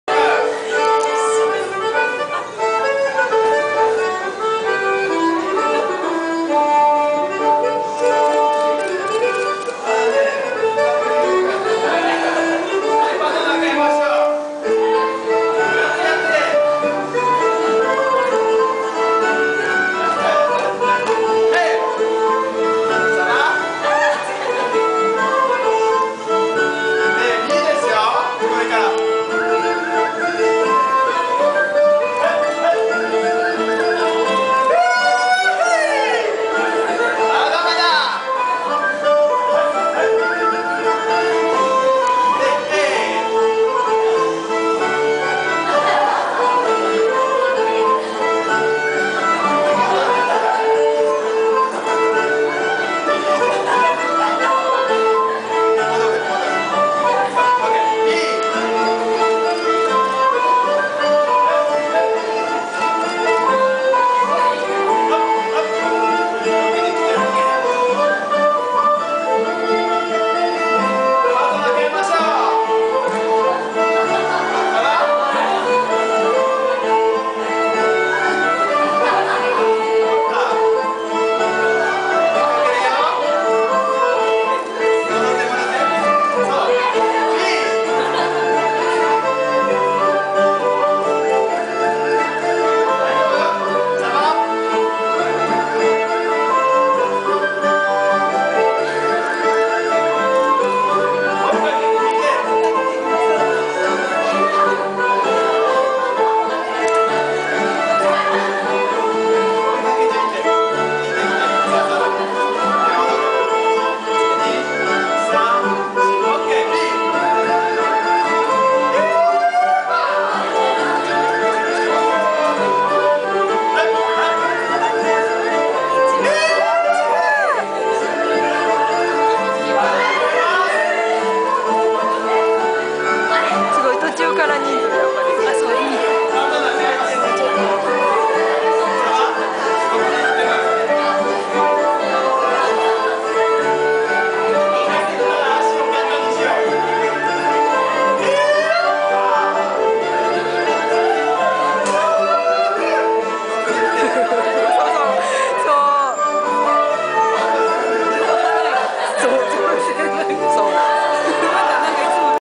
18 septembre 2011 - Kawachi Nagano World Music Festival - Bourrée en ligne
French Folk Dance (Bourrée en ligne) at Kawachi Nagano World Music Festival.